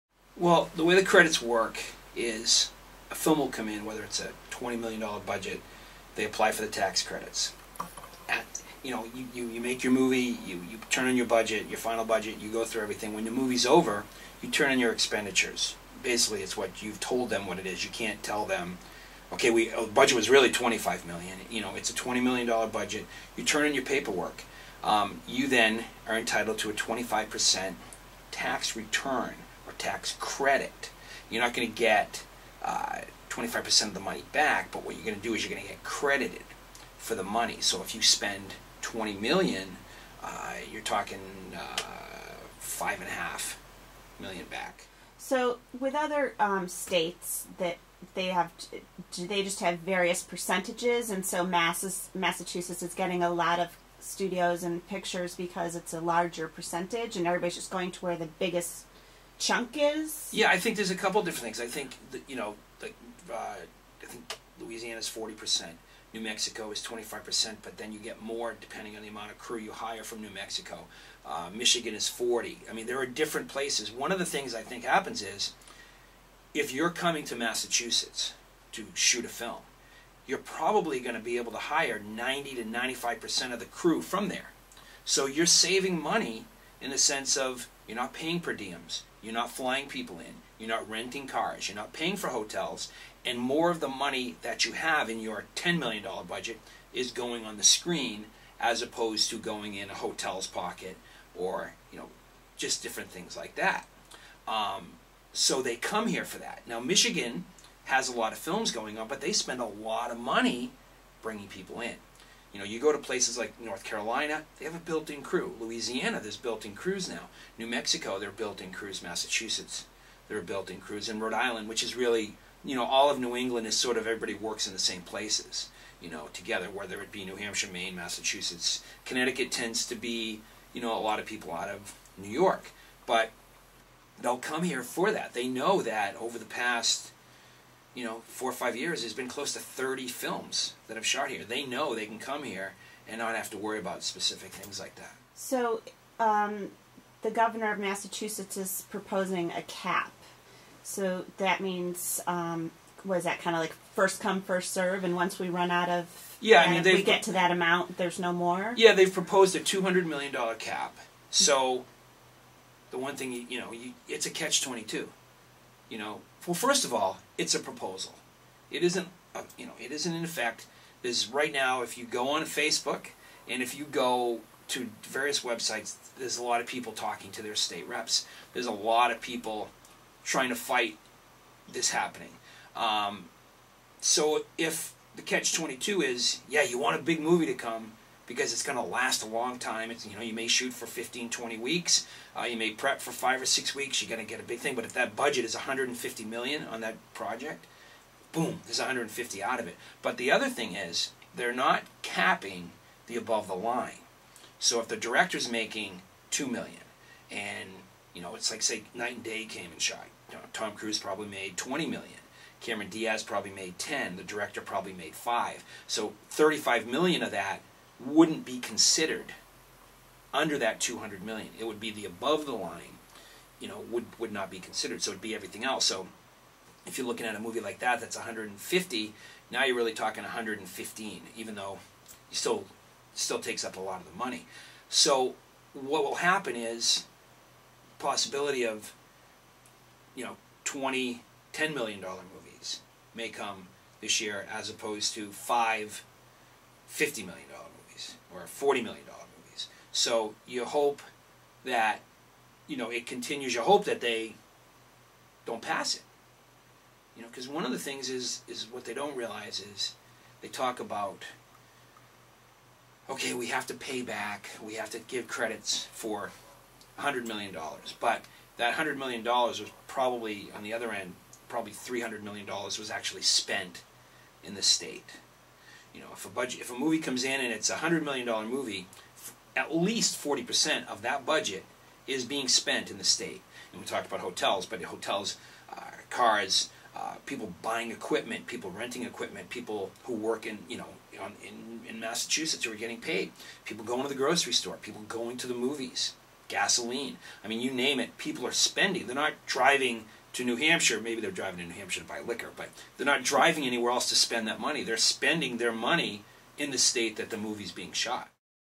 Mark Constance Part 1
Mark Constance, Executive Producer, talks about the MA Tax credits for films, how they work, and help the state.